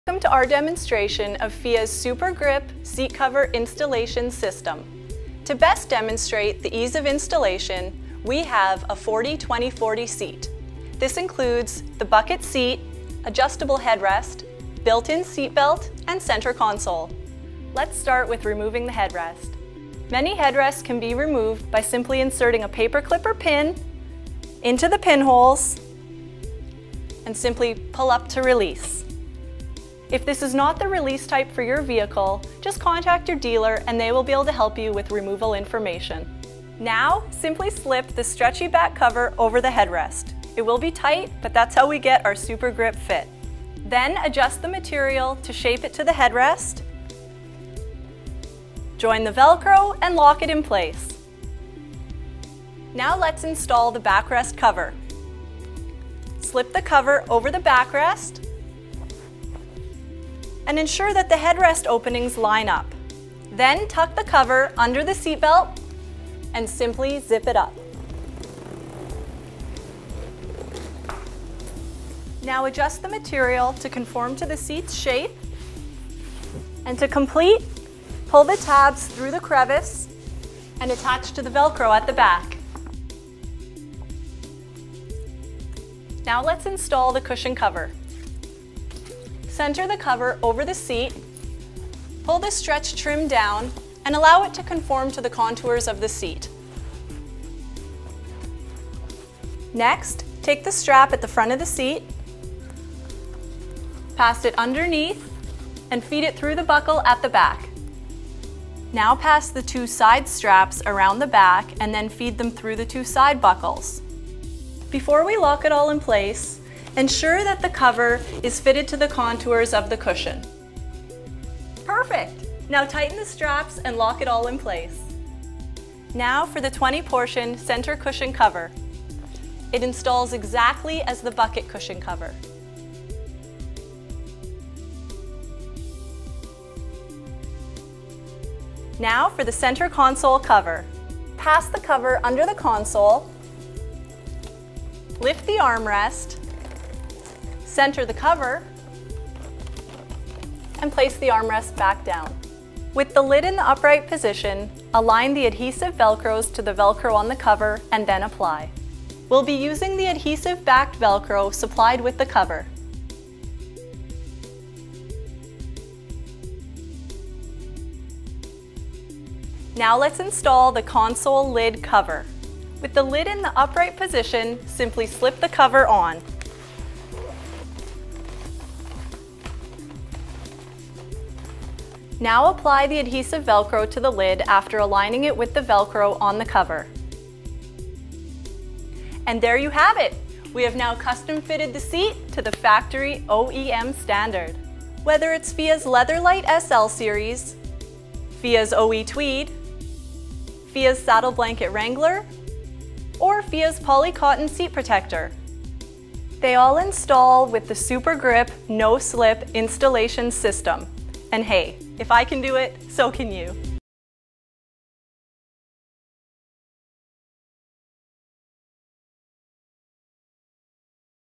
How to Install Fia Seat Covers
Fia designs their seat covers with Super-Grip installation technology which uses a combination of belts and buckles to prevent slippage. This helps to ensure a quality fit, and their extra foam padded backrests help keep them comfortable even on the longest road trips and hardest work days

Video Transcription:
Welcome to our demonstration of Fia Super-Grip seat cover installation system.

To best demonstrate the ease of installation, we have a 40/20/40 seat. This includes the bucket seat, adjustable headrest, built-in seat belt, and center console. Let's start with removing the headrest.

Many headrests can be removed by simply inserting a paperclip or pin into the pin holes, and simply pull up to release. If this is not the release type for your vehicle, just contact your dealer, and they will be able to help you with removal information.

Now simply slip the stretchy back cover over the headrest. It will be tight, but that's how we get our Super-Grip fit. Then, adjust the material to shape it to the headrest. Join the Velcro, and lock it in place.

Now let's install the backrest cover. Slip the cover over the backrest, and ensure that the headrest openings line up. Then tuck the cover under the seat belt, and simply zip it up. Now adjust the material to conform to the seat's shape. And to complete, hold the tubs through the crevice, and attach to the Velcro at the back.

Now let's install the cushion cover. Center the cover over the seat, pull the stretch trim down, and allow it to conform to the contours of the seat. Next, take the strap at the front of the seat. Pass it underneath, and feed it through the buckle at the back. Now pass the two side straps around the back, and then feed them through the two side buckles.

Before we lock it all in place, ensure that the cover is fitted to the contours of the cushion.

Perfect. Now tighten the straps, and lock it all in place.

Now for the 20 portion center cushion cover. It installs exactly as the bucket cushion cover.

Now for the center console cover. Pass the cover under the console, lift the armrest, center the cover, and place the armrest back down. With the lid in the upright position, align the adhesive Velcros to the Velcro on the cover and then apply. We'll be using the adhesive back Velcros supplied with the cover.

Now let's install the console lid cover. With the lid in the upright position, simply slip the cover on. Now apply the adhesive Velcro to the lid after aligning it with the Velcro on the cover.